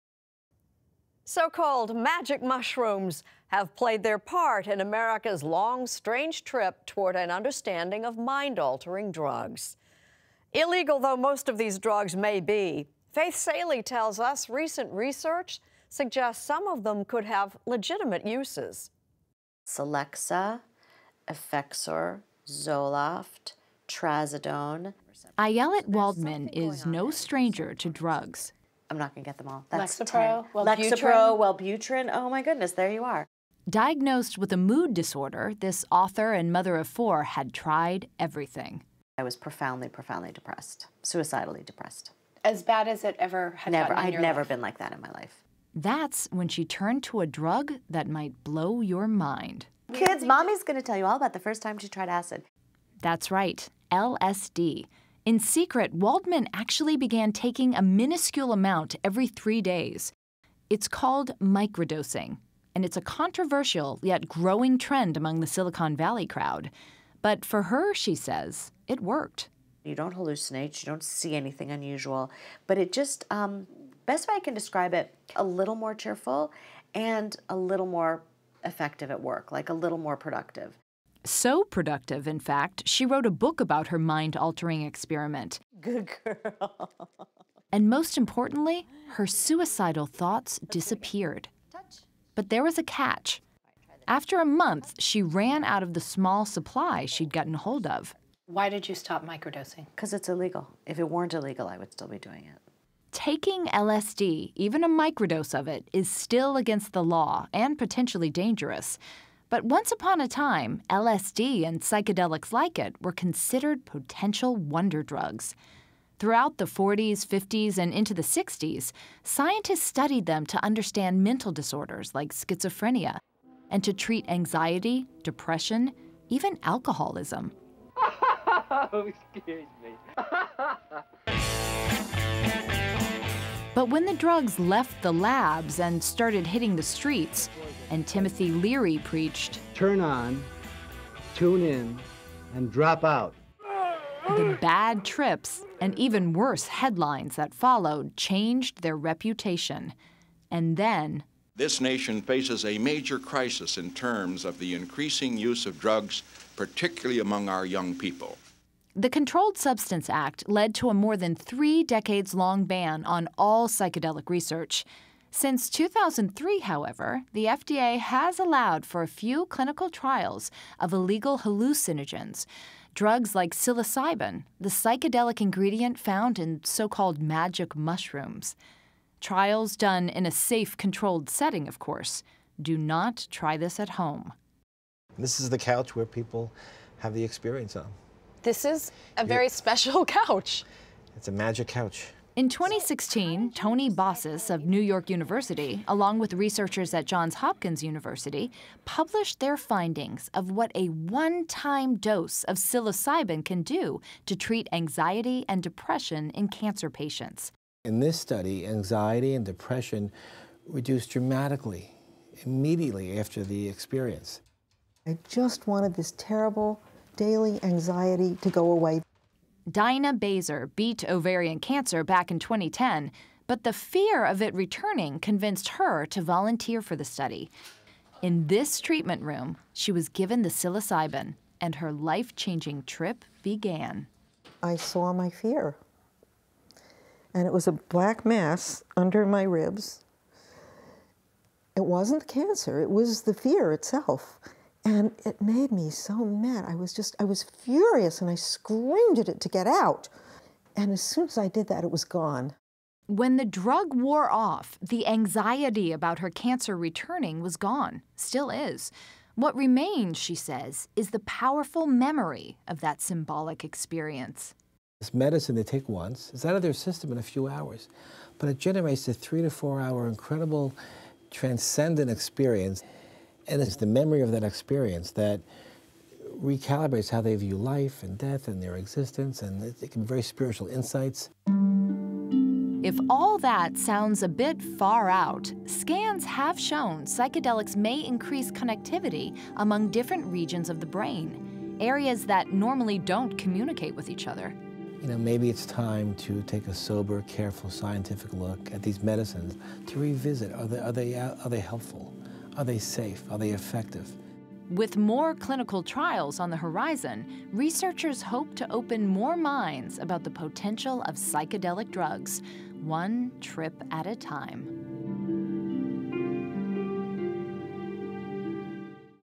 Altered states: Can LSD contribute to mental health?
Faith Salie reports on how LSD and "magic mushrooms" are being studied for their ability to help patients conquer anxiety and depression without the psychedelia long associated with mind-altering drugs. Salie talks with author Ayelet Waldman, who microdosed LSD and overcame a deep depression, and with NYU professor Tony Bossis, who conducted a clinical trial of cancer patients treated for depression with a one-time dose of the psychedelic Psilocybin.